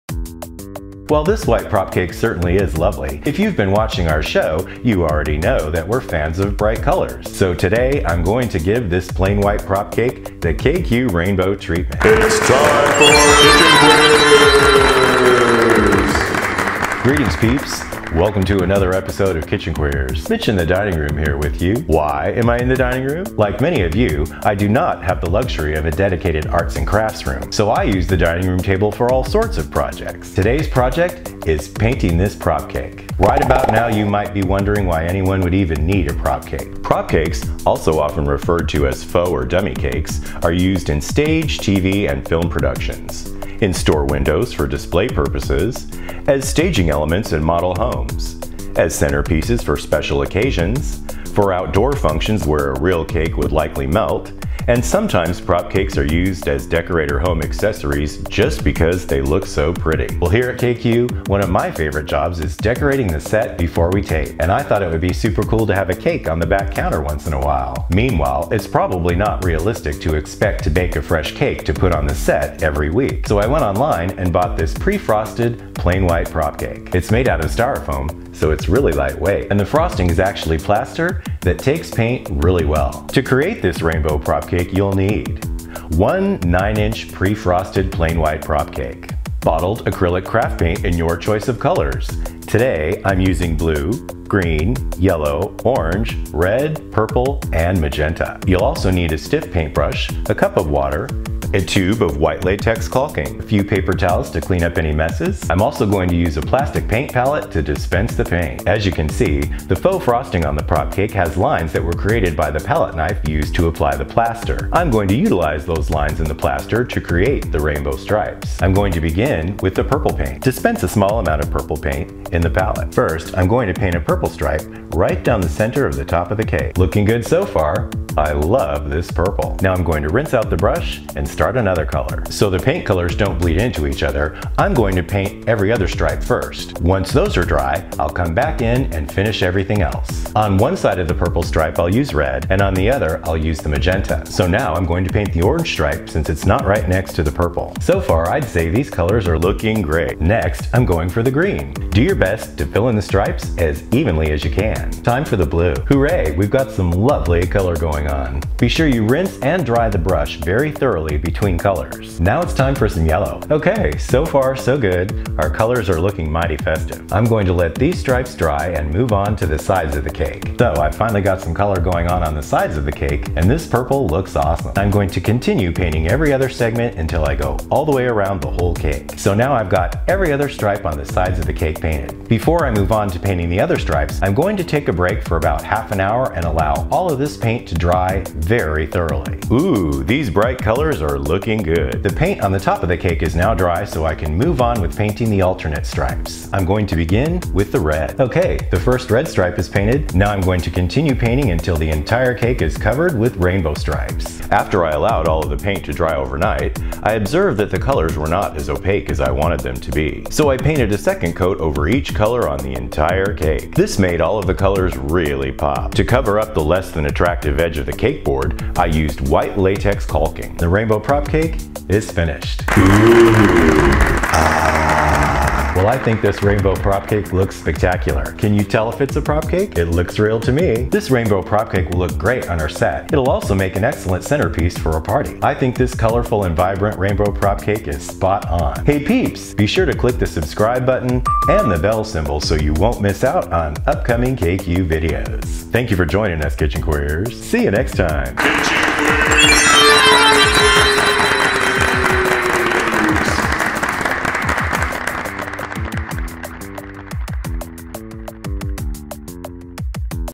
Rainbow Prop Cake
In this special "arts and crafts" video, KQ host Mitch Hightower takes a plain white PropCake and uses acrylic paint to give it the KQ rainbow treatment. Mitch also explains many practical uses for FauxCakes. KQ Season 5, Episode 15, Number 101, (KQ-S5E15N101-HD).